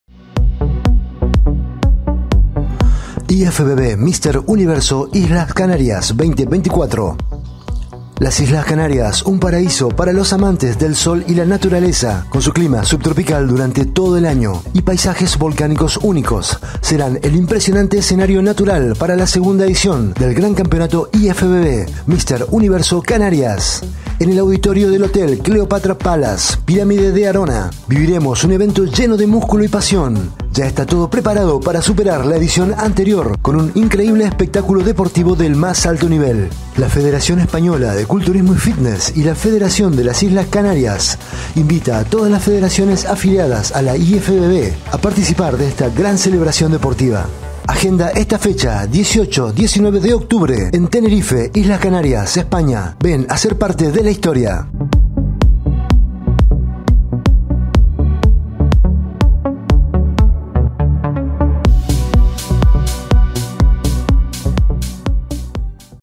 MR UNIVERSO ISLAS CANARIAS 2024
Las Islas Canarias, un paraíso para los amantes del sol y la naturaleza con su clima subtropical durante todo el año y sus paisajes volcánicos únicos, serán el impresionante telón de fondo para la segunda edición del gran campeonato de la IFBB, Señor Universo.

En el Auditorio del Hotel Cleopatra Palace - Pirámide de Arona, viviremos un evento lleno de músculos preparado para superar la edición anterior con un increíble espectáculo deportivo de primer nivel.

Marca tu calendario: 18-19 de octubre en Tenerife, Islas Canarias, España. Ven y sé parte de la historia.